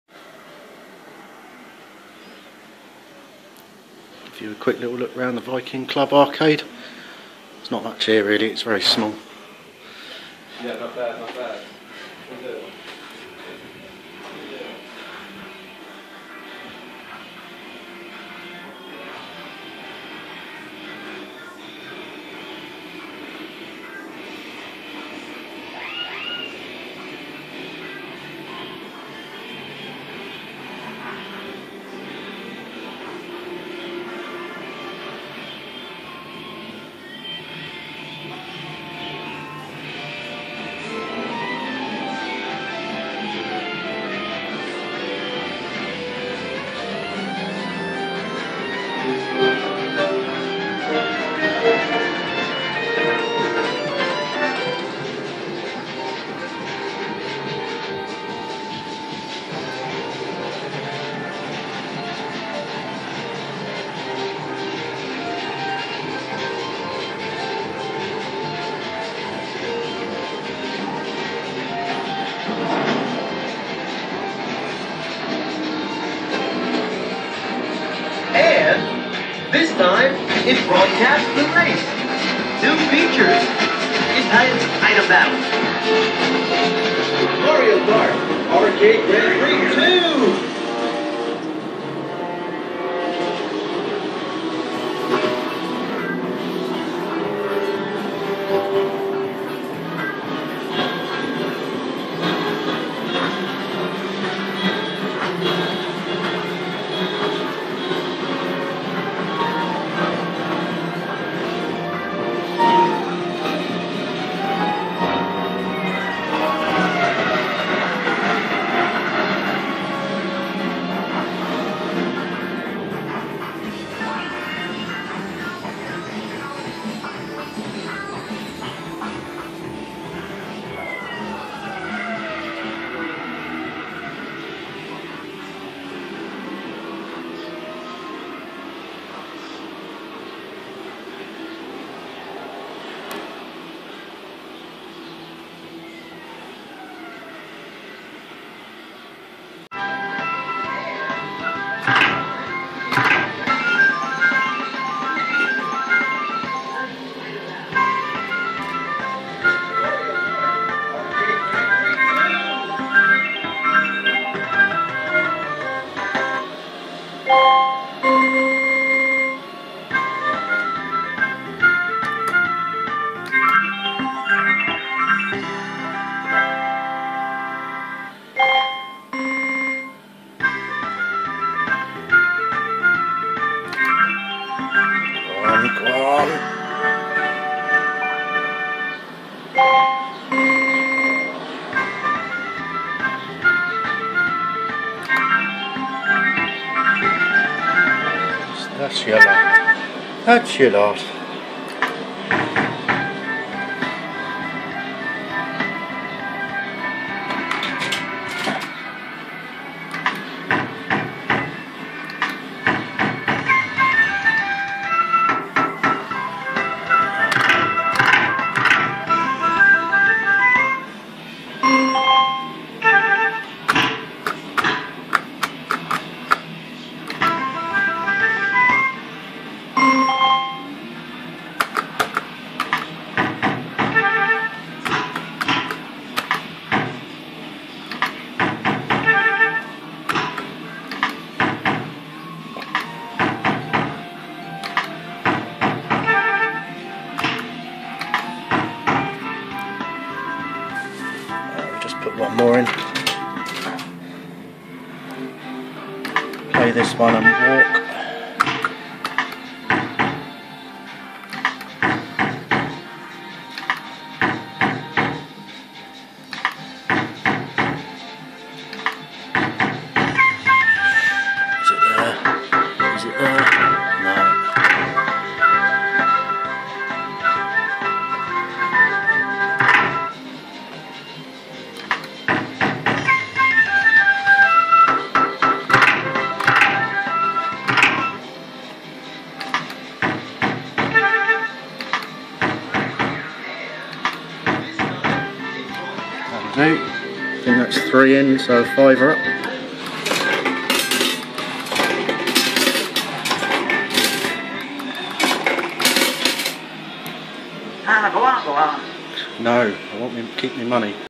The Viking Club Arcade Tour at Green Lawns Bunn Leisure Selsey 2016
A look round the small arcade at the Viking Club,  then a quick go on Lady Luck Fruit Machine as id done well on it in the past :)